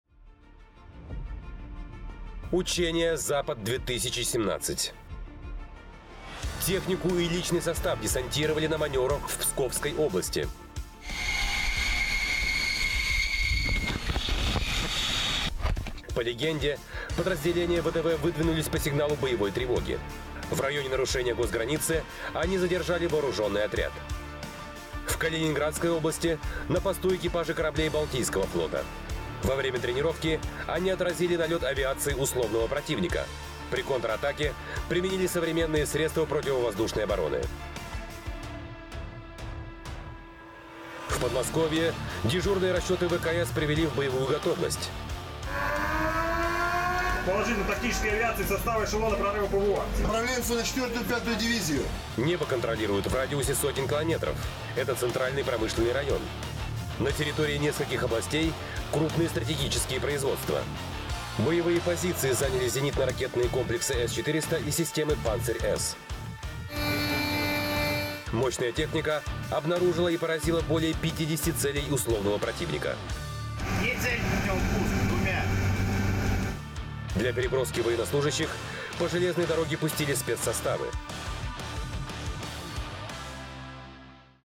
Учения «Запад-2017»
Технику и личный состав десантировали в Псковской области